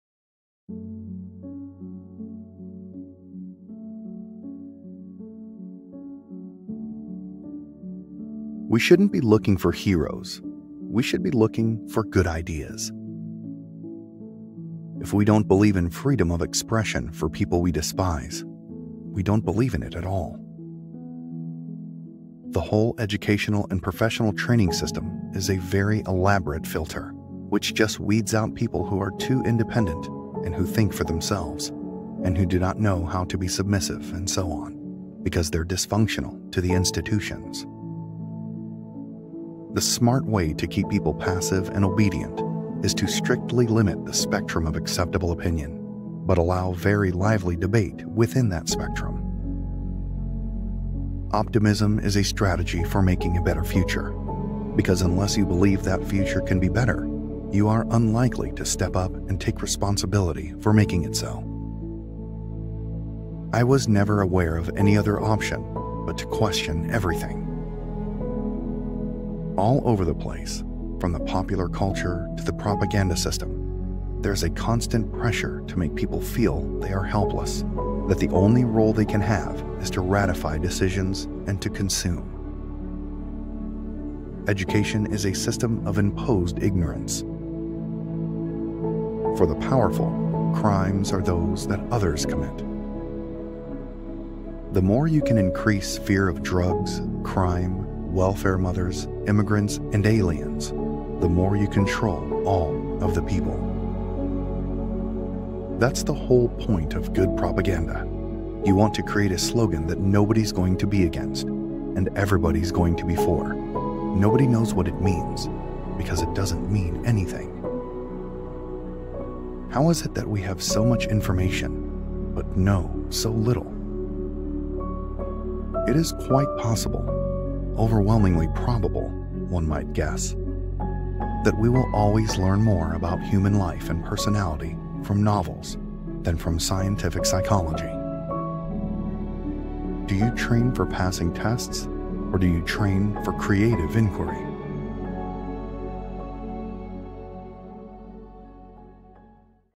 Noam Chomsky - Quotes (Audio)
Quotes Out Loud
Writer Series presents
Noam Chomsky
(Audio Read Acted Quotes Out Loud)

We shouldn't be looking for heroes, we should be looking for good ideas

If we don't believe in freedom of expression for people we despise, we don't believe in it at all

The whole educational and professional training system is a very elaborate filter, which just weeds out people who are too independent, and who think for themselves, and who don't know how to be submissive, and so on -- because they're dysfunctional to the institutions

The smart way to keep people passive and obedient is to strictly limit the spectrum of acceptable opinion, but allow very lively debate within that spectrum

Optimism is a strategy for making a better future. Because unless you believe that the future can be better, you are unlikely to step up and take responsibility for making it so

I was never aware of any other option but to question everything

All over the place, from the popular culture to the propaganda system, there is constant pressure to make people feel that they are helpless, that the only role they can have is to ratify decisions and to consume

Education is a system of imposed ignorance

For the powerful, crimes are those that others commit

The more you can increase fear of drugs, crime, welfare mothers, immigrants and aliens, the more you control all of the people

That's the whole point of good propaganda. You want to create a slogan that nobody's going to be against, and everybody's going to be for. Nobody knows what it means, because it doesn't mean anything

How it is we have so much information, but know so little?

It is quite possible--overwhelmingly probable, one might guess--that we will always learn more about human life and personality from novels than from scientific psychology

Do you train for passing tests or do you train for creative inquiry?